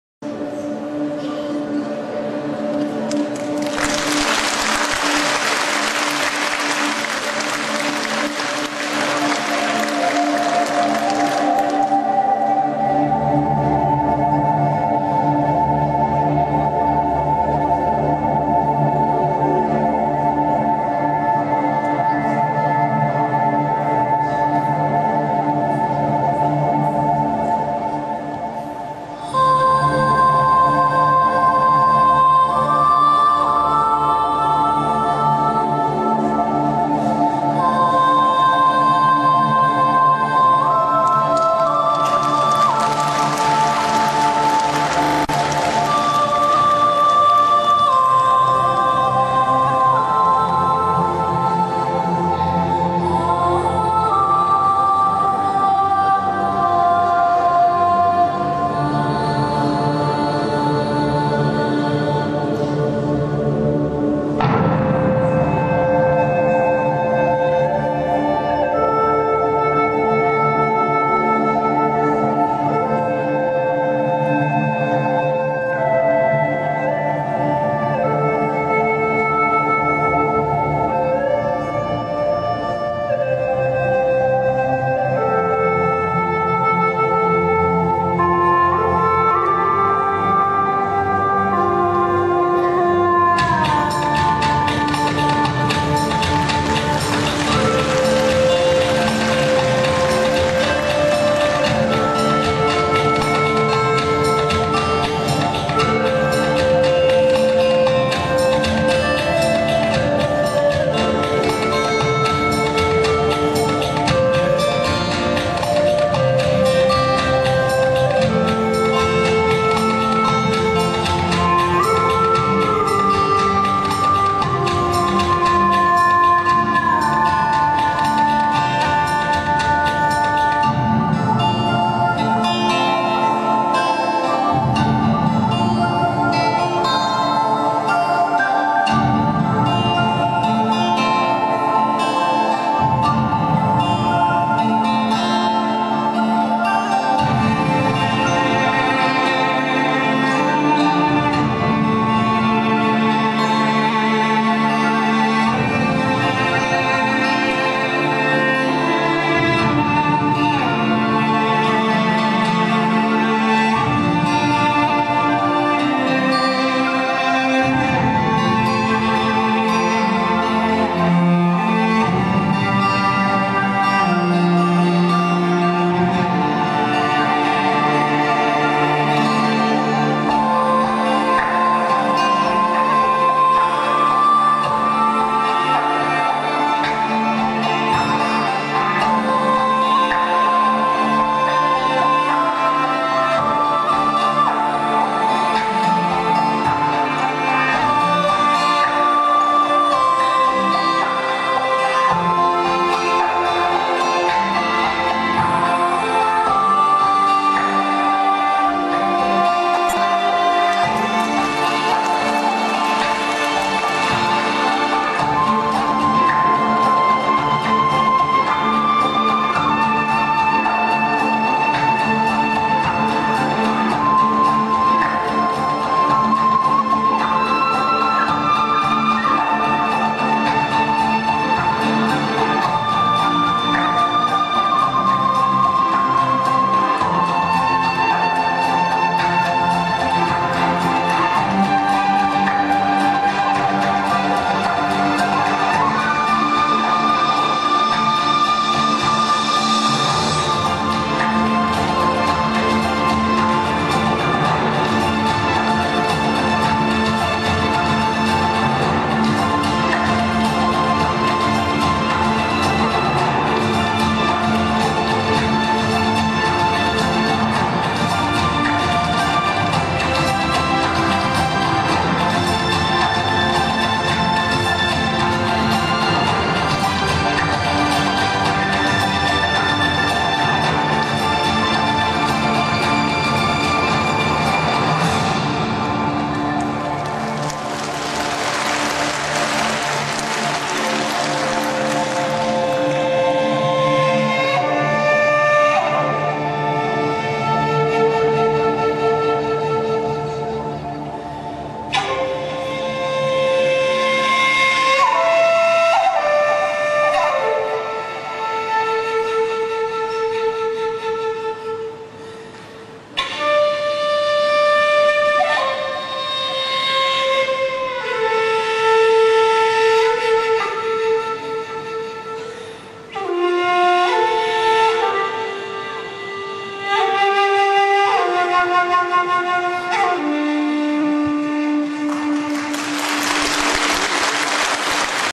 Танец «Лунный свет» – Ян Липин, Китай
Легендарная китайская танцовщица Ян Липин исполняет поистине магический «лунный танец».
Света первого сестра,
Образ нежности в печали,
Вкруг тебя туманы встали,
Как фата из серебра.
Поступь лёгкую твою
Слышит всё, что днём таится.
Чуть вспорхнёт ночная птица,
Грустный призрак, я встаю...

И. Гёте
(пер. В. Левика)

Аполлинариум. Жизнь коротка, искусство вечно.